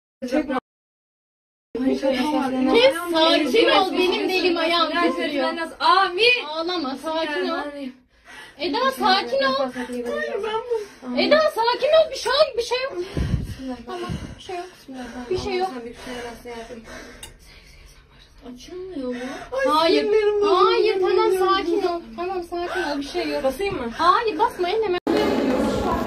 Yine KYK yine asansör faciası! Asansördeki panik anları kamerada
Ordu'nun Fatsa ilçesinde Kredi Yurtlar Kurumu'na (KYK) bağlı yurtta arıza nedeniyle 4 öğrenci, asansörde mahsur kaldı. Öğrenciler arkadaşları tarafından kurtarılırken, boşaltılan asansör bodrum kata çakıldı. 4 öğrenci sinir krizi geçirirken, olay ile ilgili soruşturma başlatıldı.

Olay, dün saat 22.00 sıralarında ilçeye bağlı Efkaf Mahallesi'ndeki KYK'ya bağlı öğrenci yurdunda meydana geldi. Yurtta kalan 4 öğrenci, 5'inci kattan bindikleri asansör ile aşağı inmek istedi. Ancak asansör 3'üncü katta arızalanarak durdu, öğrenciler mahsur kaldı. Sesleri duyup, yardım koşan diğer öğrenciler, arkadaşlarını kapıyı açarak asansörden kurtardı. 5 saniye sonra da asansör hızlı bir şekilde bodrum kata çakıldı. Mahsur kalıp, sinir krizi geçiren 4 öğrenci ambulanslarla Fatsa Devlet Hastanesi'ne götürüldü. 4 öğrenci yurt yönetiminden şikayetçi olurken, olayla ilgili soruşturma başlatıldı. Öte yandan öğrenciler, yurt yönetimini protesto etti. (DHA)